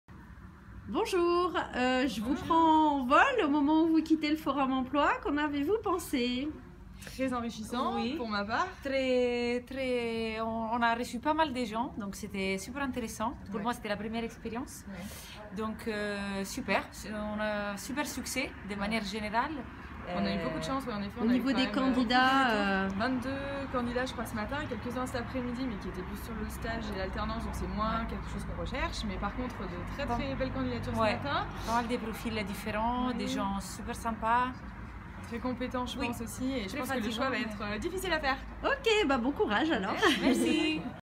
Alessandra et Karine de chez Mhikes
Quelques mots à la sortie du Forum Emploi d'inovallée 2018